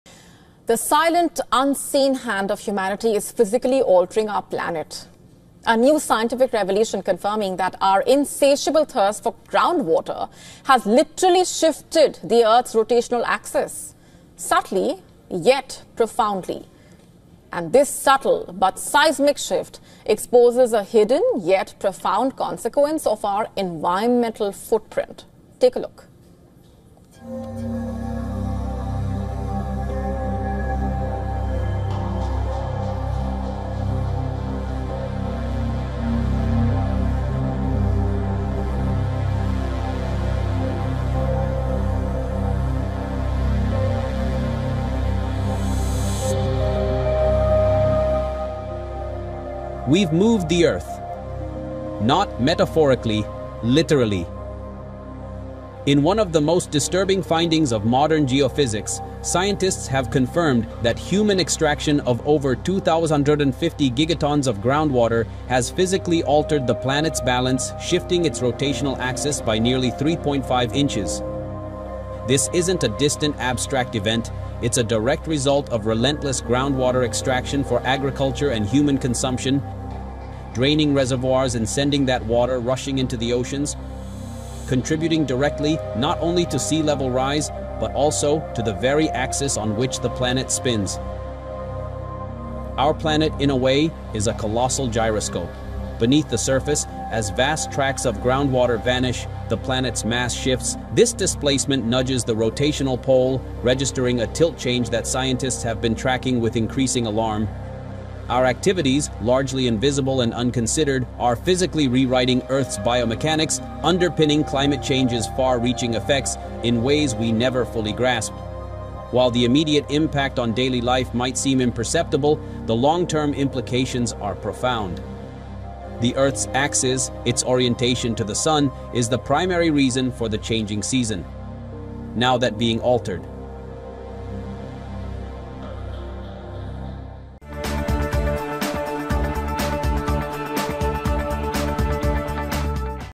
Our Thirst for Groundwater is Physically Tilting the Planet! | Gravitas
In a groundbreaking scientific discovery, Earth has physically tilted 3.5 inches due to excessive groundwater extraction. With over 2,150 gigatons of mass redistributed, what does this mean for our future? The answers are more chilling than you think. Watch now.